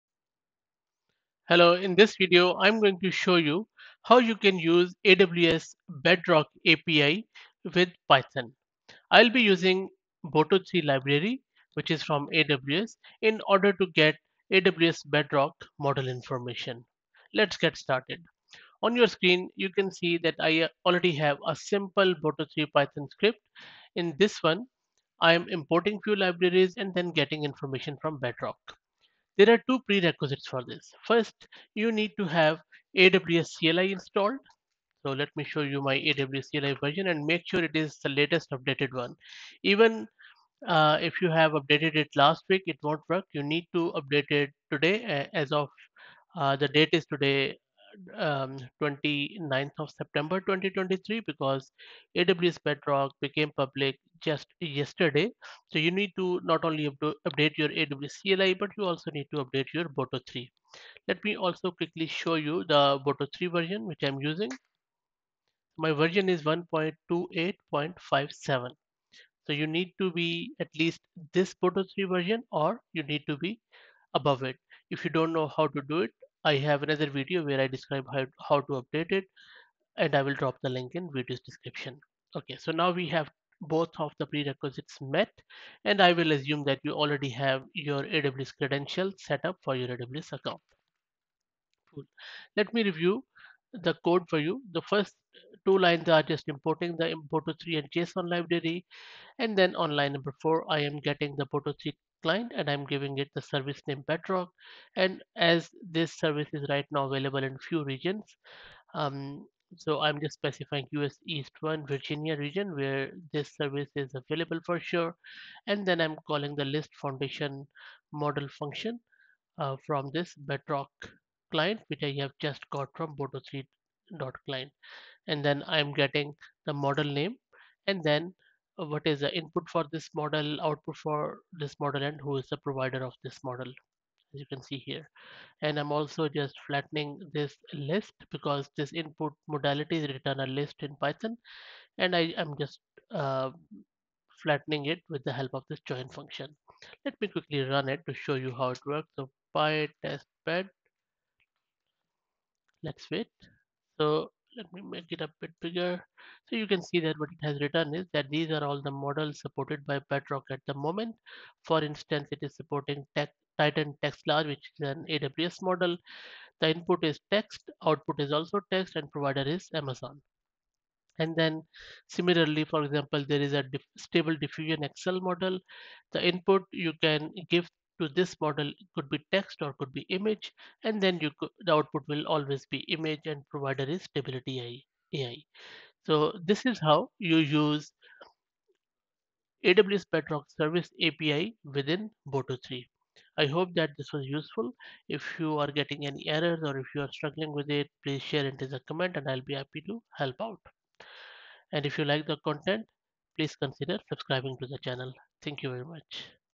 How To Use AWS Bedrock API in Boto3
This video shows code as how to use Boto3 with Amazon Bedrock API.